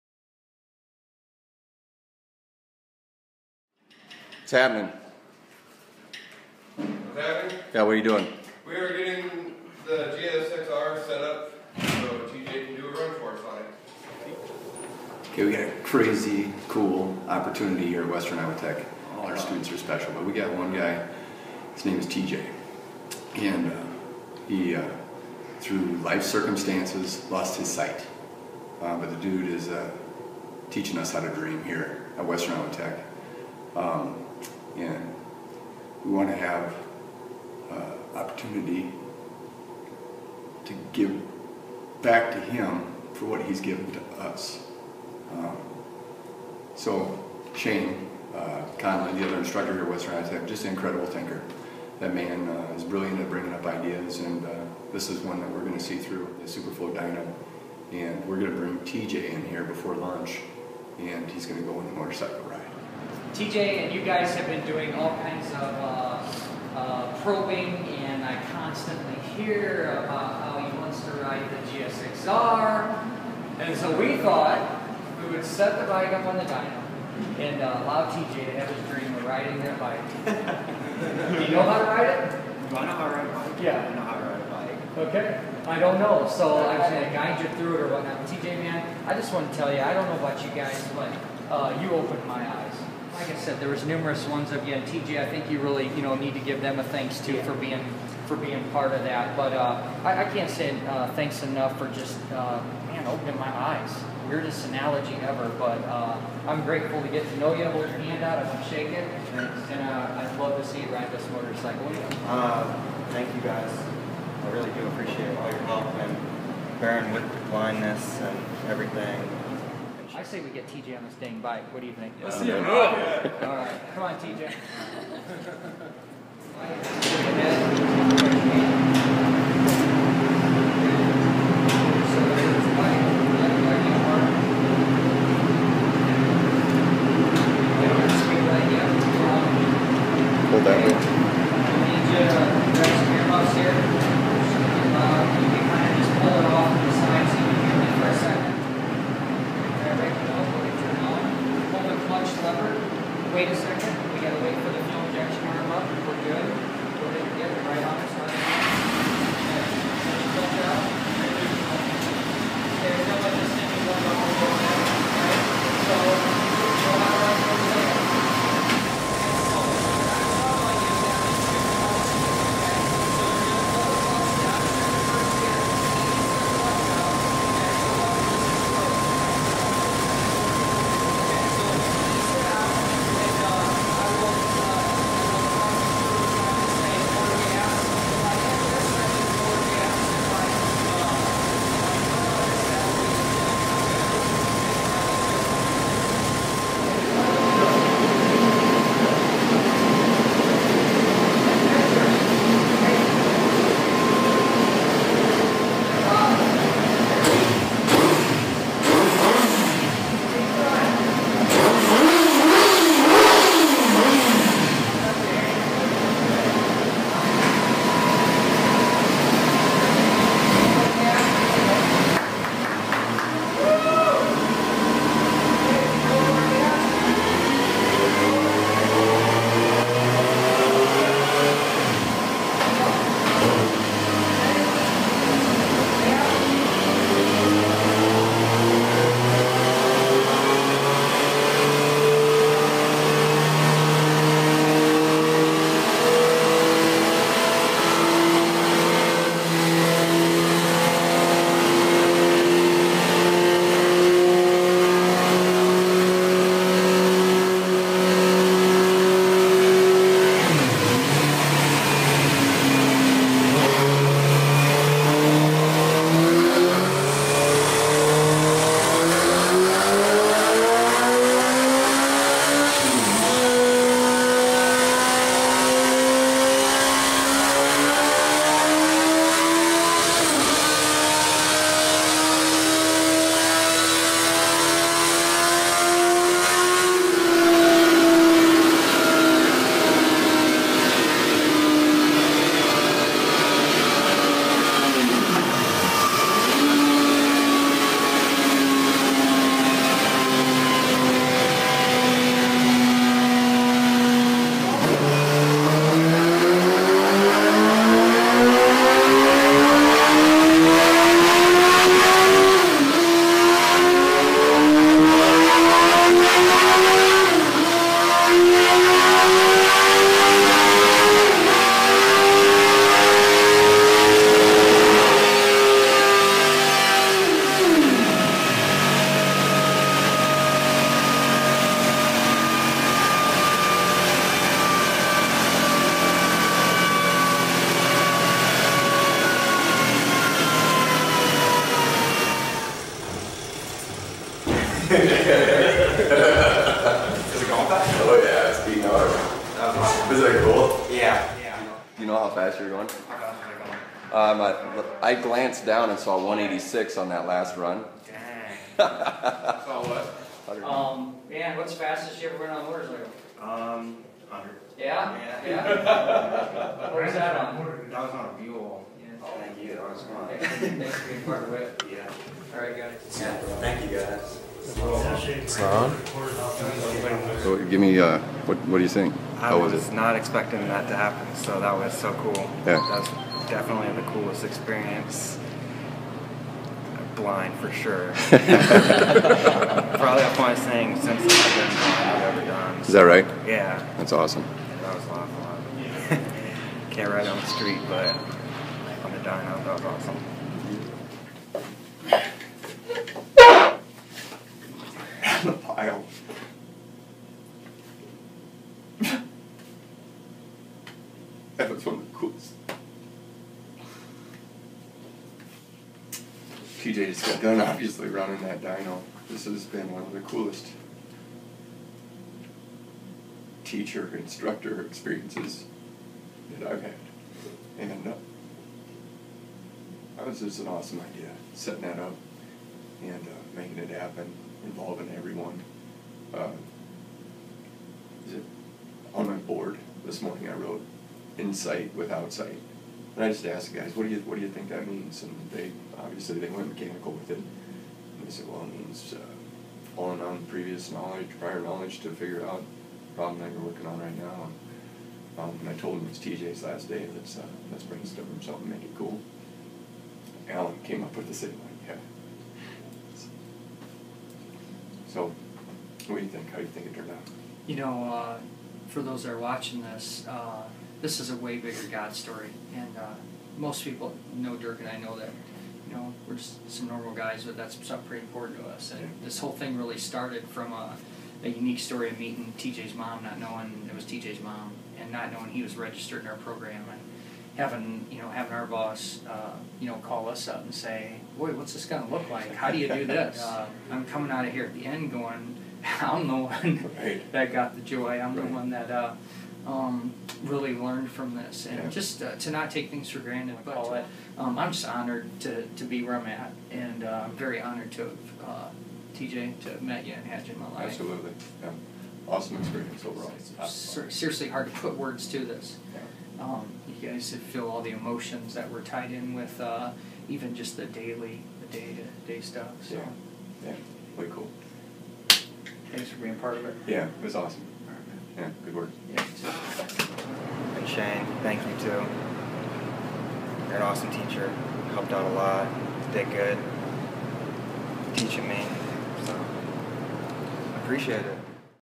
Blind man riding a motorcycle on the dyno for his last day at motorcycle mechanics school 186 MPH!
This video was filmed in the lab at Western Iowa Tech Community College Motorcycle and Powersports Technology Program. In 2014 I was approached by the administration about a blind man wanting to join the program. With so much safety and liability at hand there were a lot of concerns on "how could you possibly teach a blind man mechanics"? I really had no idea but after meeting TJ in person, I connected with him and thought there is no way we can say no to a man with more determination than most traditional students I meet. Here is a brief look into a fun time we had saying goodbye to TJ.
I swear I learned more about the human heart and the will to never give up, than anything I could have possible taught TJ...
Forever Grateful,
Keep Wrenching,
Coach Conley